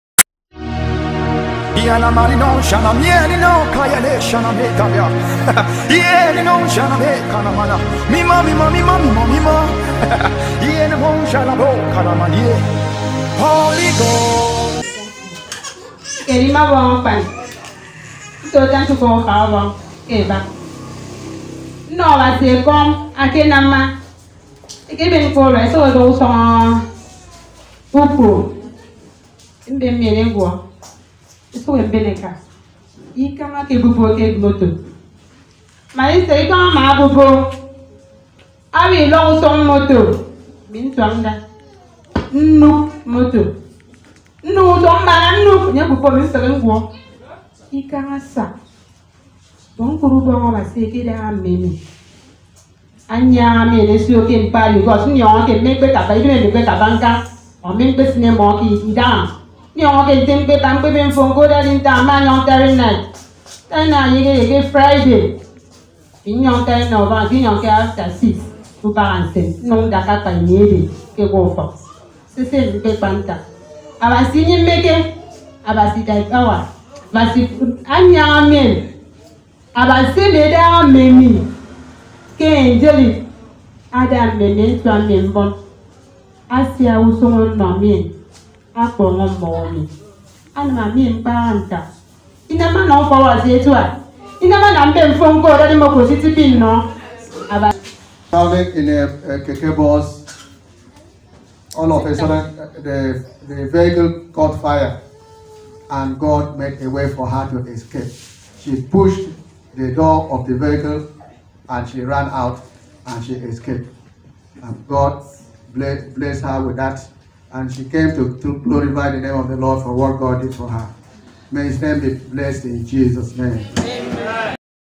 Testimony of Deliverance from Fire Outbreak in a Vehicle she was but she Escaped - 18/Oct/2020
She went to pick her goods yesterday at Itam Market which she successfully picked but the vehicle she was caught fire and the Lord rescued her immediately and she is alive to testify. Praise God. Amen.